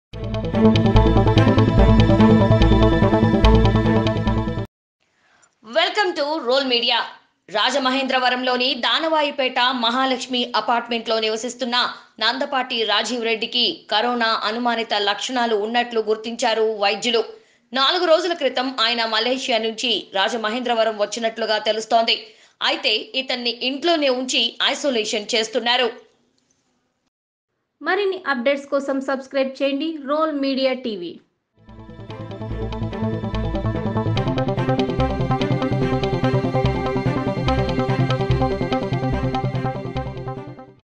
Corona Virus Updates: రాజమహేంద్రవరంలో కరోనా కలకలం | Role Media Tv
Description:
రాజమహేంద్రవరంలోని దానవాయిపేట మహాలక్ష్మి అపర్ట్మెంట్లో నివసిస్తున్న నందపాటి రాజీవ్ రెడ్డికి కరోనా అనుమానిత లక్షణాలు ఉన్నట్లు గుర్తించారు వైద్యులు.. నాలుగు రోజుల క్రితం ఆయన మలేసియా నుంచి రాజమహేంద్రవరం వచ్చినట్లుగా తెలుస్తోంది. అయితే అతన్ని ఇంట్లోనే ఉంచి ఐసోలేషషన్ చేస్తున్నారు

Role Media TV is the Telugu Channel to bring all the Updates of Telugu World to your hand.